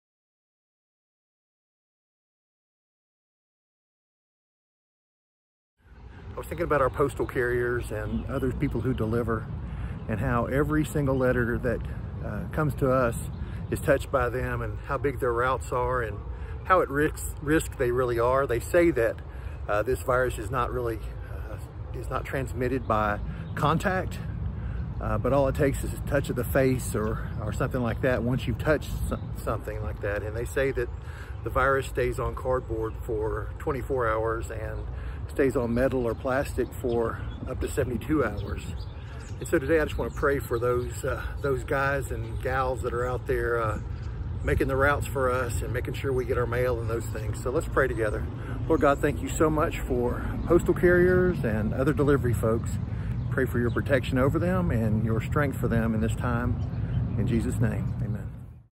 Just a Minute: Episode One
Just a Minute to give you something to pray about. Praying for those who deliver mail and parcels.